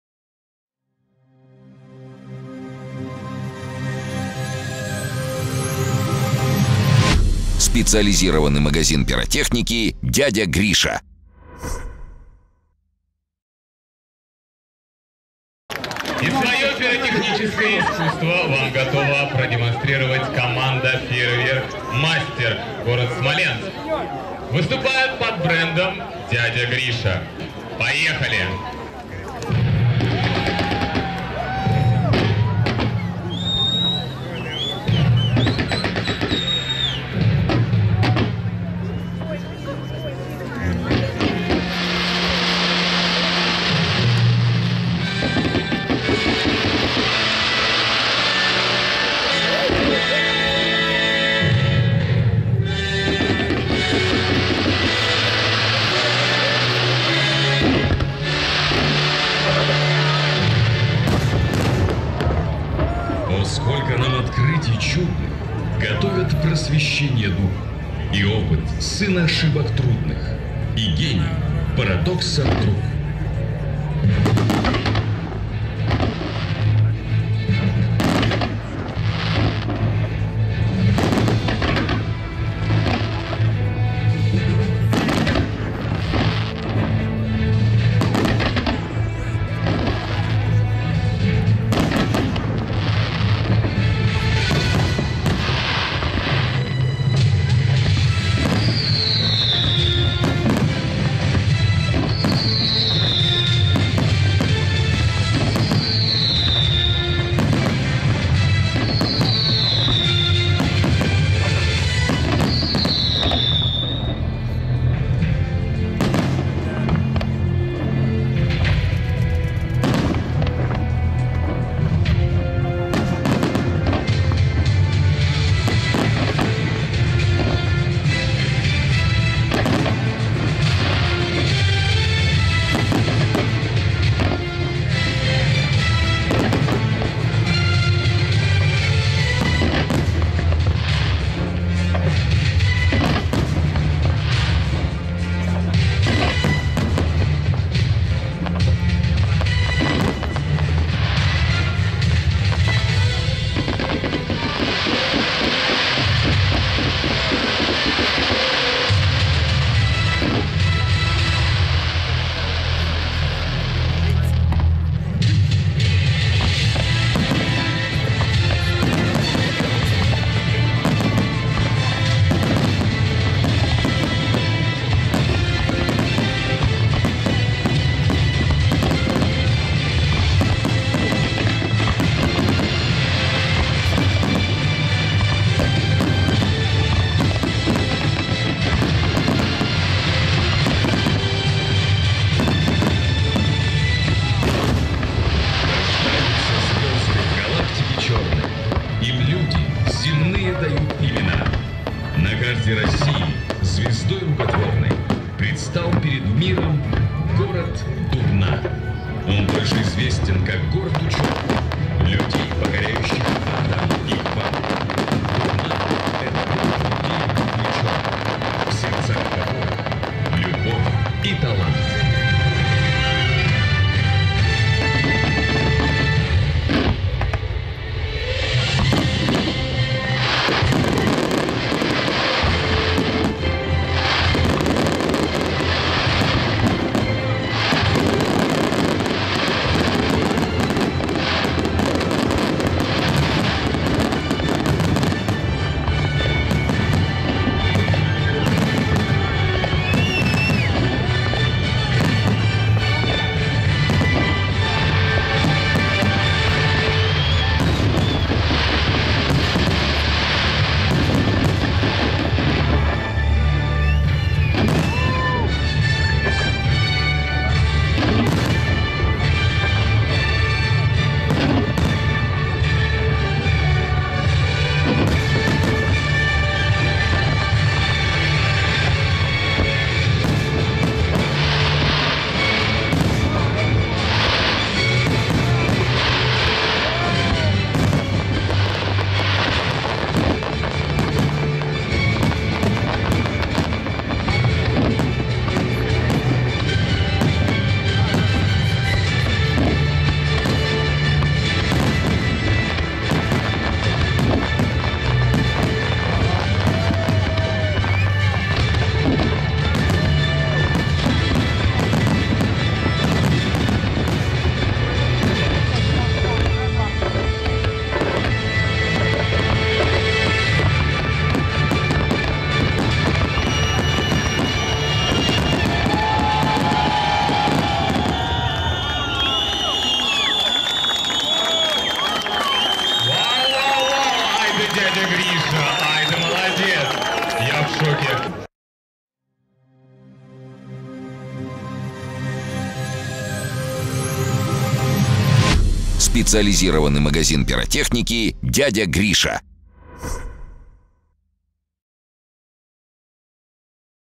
Фестиваль фейерверков в Дубне 2016
Фестиваль фейерверков в "Большая Волга" г. Дубна. Специализированный магазин пиротехники "Дядя Гриша"
Примерная стоимость - 350 000 руб.